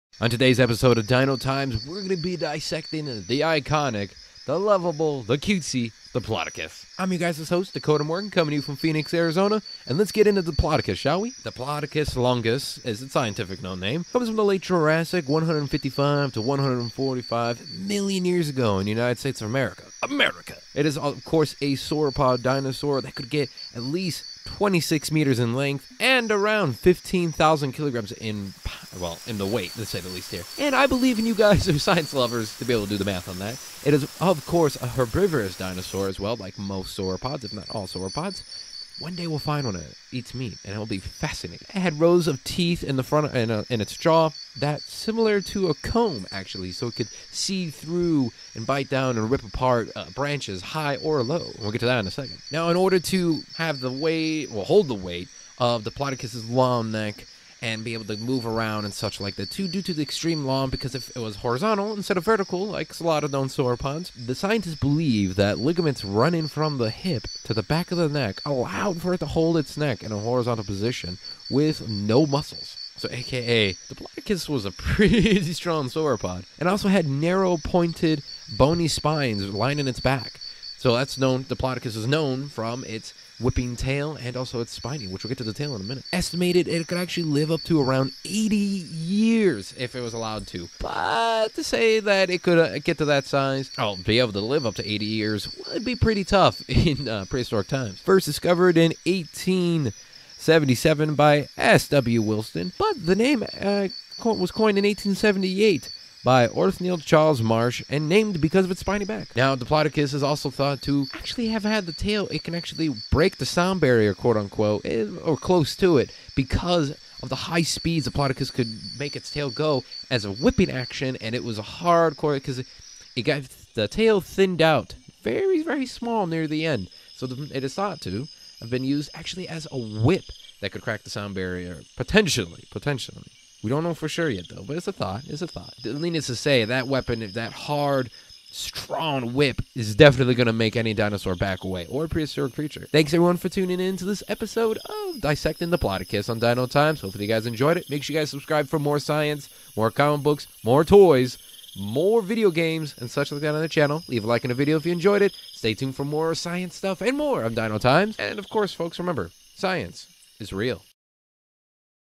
Dissecting Diplodocus! Dino Times.
In this episode of Dino Times, I dissect and go over the iconic sauropod, Diplodocus!